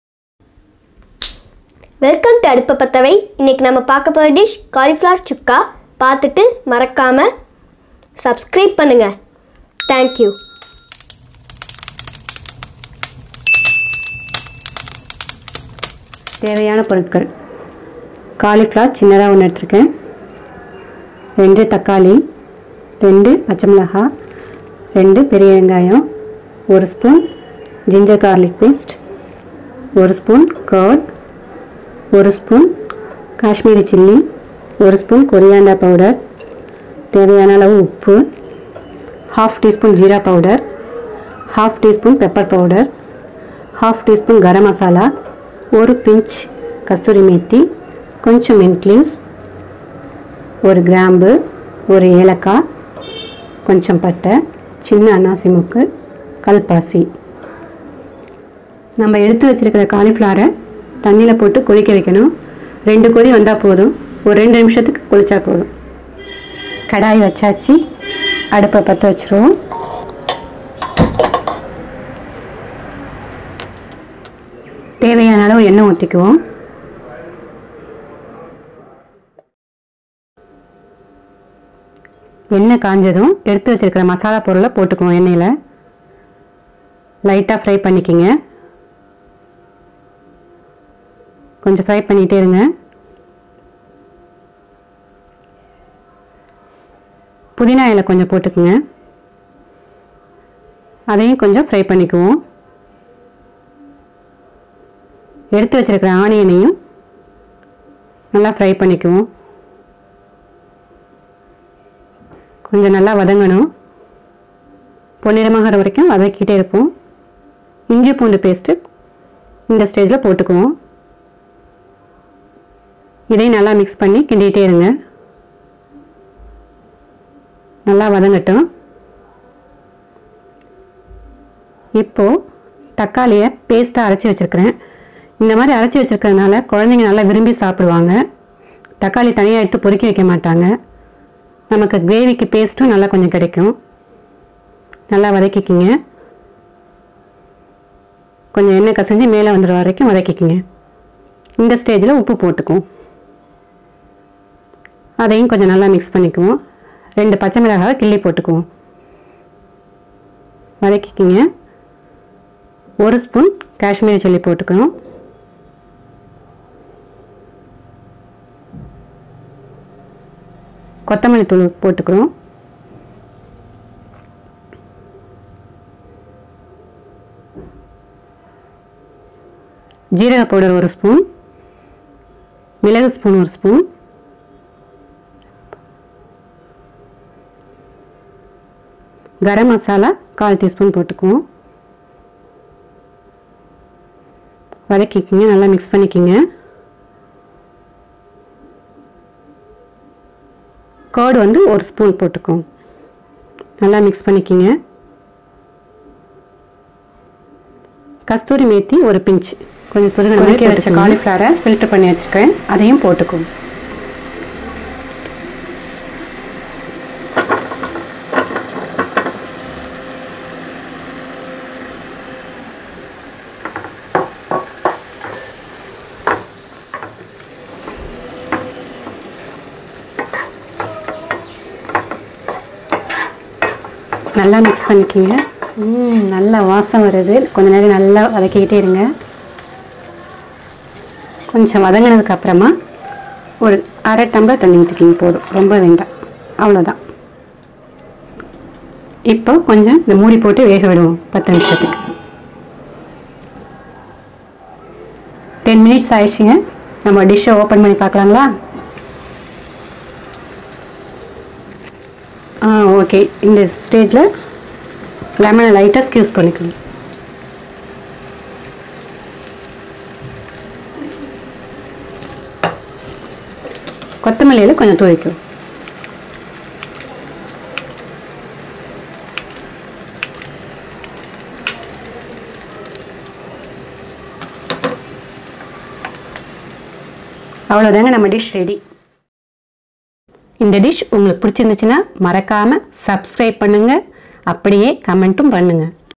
How to make gobi chukka | Cauliflower recipe in tamil
How to make cauliflower chukka in tamil.
காலிபிளவர் சுக்கா செய்வது எப்படி ?
காலிஃப்ளவர் ஃப்ரை செய்வது எப்படி?
how to make gobi fry?
how to make gobi Manchurian chukka in tamil?
This episode showcases the youtube audiences with the method to make gobi recipes / cauliflower recipes in tamil.

INGREDIENTS:
1. Cauliflower
2. Onion
3. Tomato
4. Green chilly
5. Kashmiri Chilly powder
6. Corriander powder
7. Pepper powder
8. Jeera powder
9. Garam masala
10. Curd
11. Kasturi methi
12. Ginger garlic paste
13. Cinnamon
14. Clove
15. Cardamom
16. Black stone
17. Star Anise
18. Corriander leaves
19. Cooking Oil